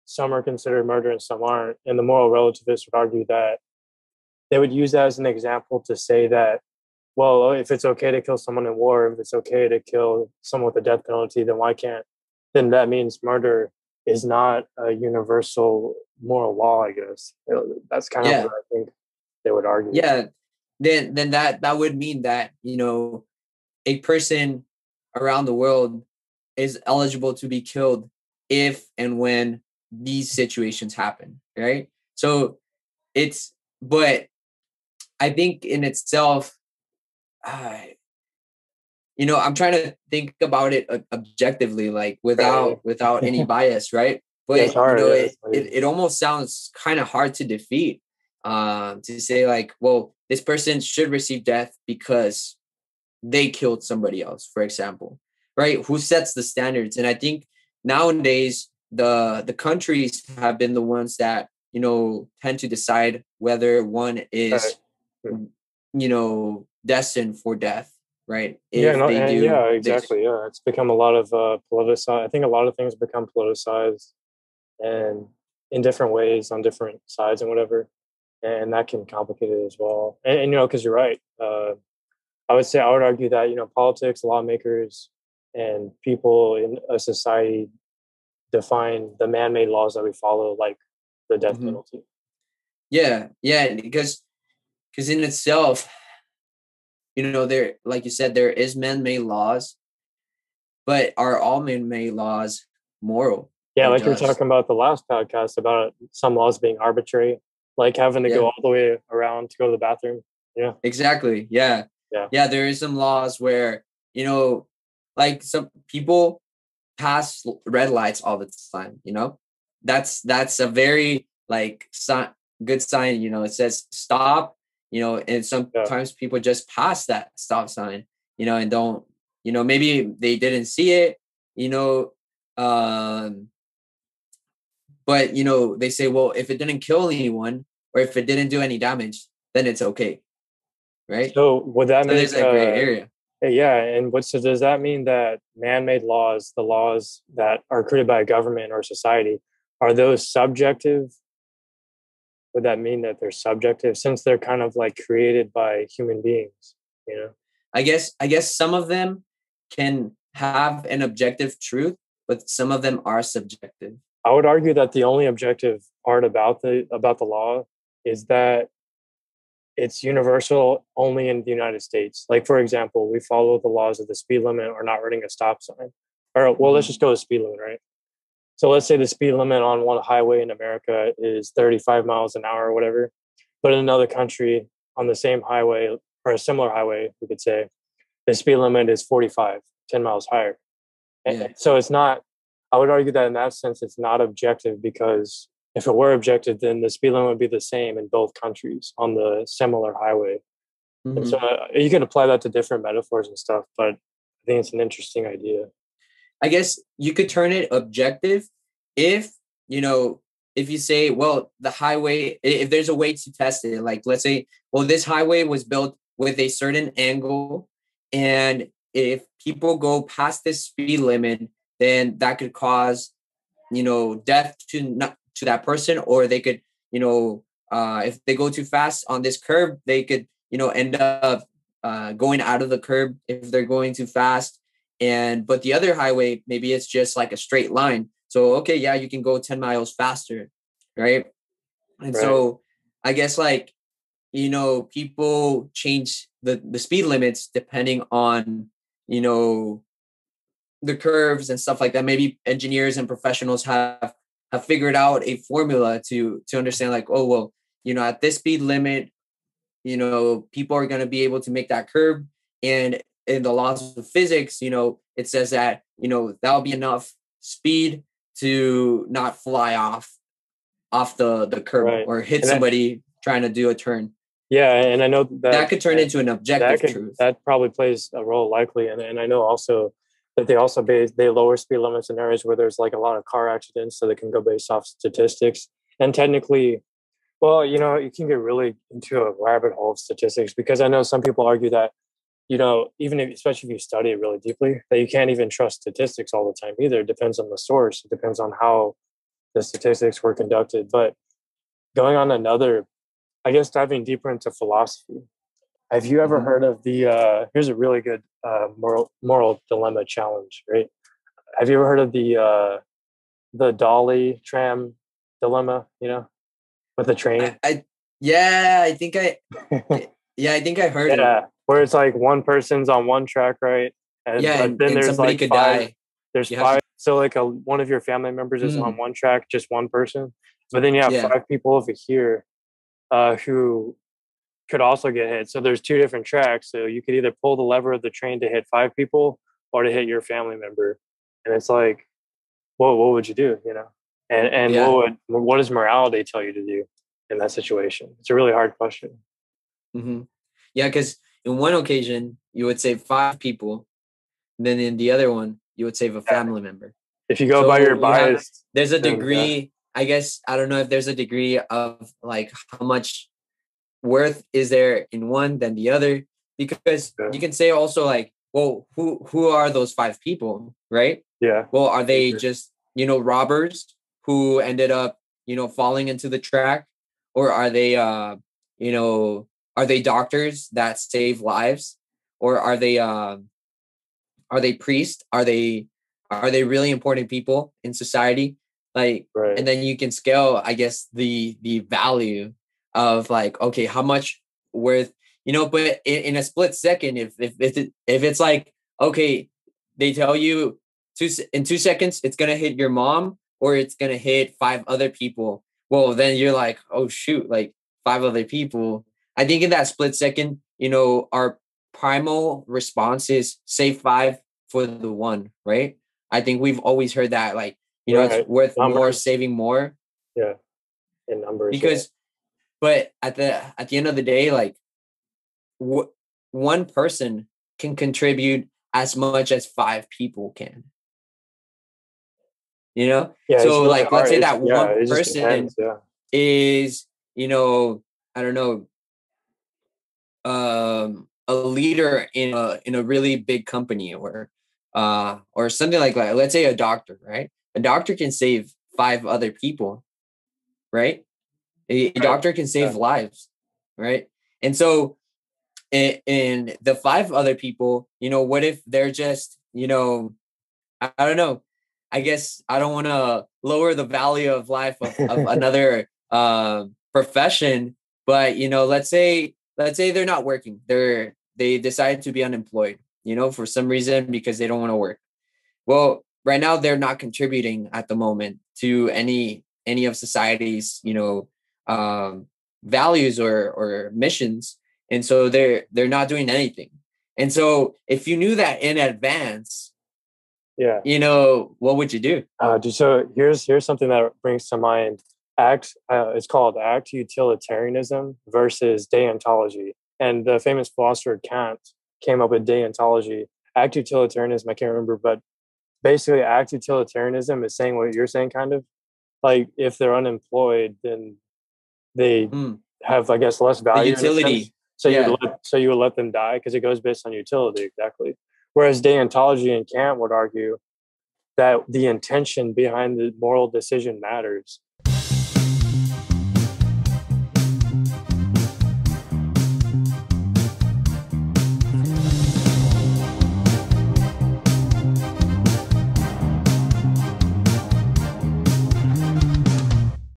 OutoftheBox Podcast #3 | Relative Morality Dilemma (Preview)
A preview of our 3rd OutoftheBox Podcast that can be viewed in FULL on our OTHER YouTube channel, Out Of The Box

Audio Edited by: Jose Tapia
Video Edited by: Steal Adcock